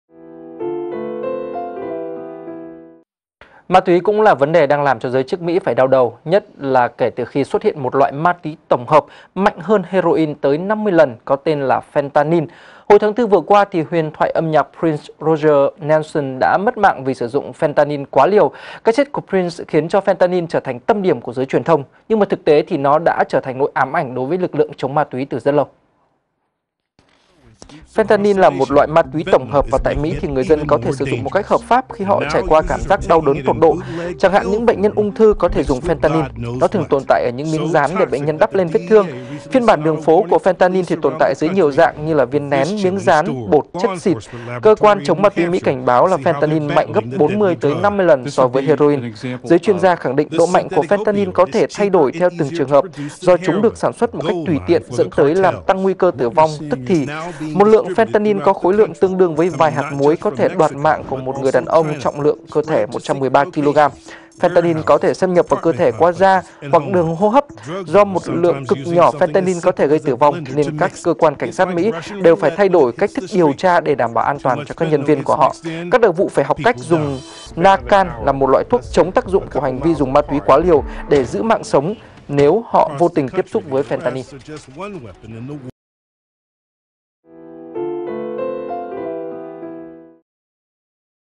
Loại ma túy khiến cảnh sát Mỹ hết hồn | VTC1
VTC1 | Nguy hiểm hơn heroin tới 50 lần, ma túy tổng hợp fentanyl đang trở thành nỗi ám ảnh của cảnh sát và các lực lượng chống ma túy ở Mỹ.
Xem bản full tại đây: goo.gl/nCbpBm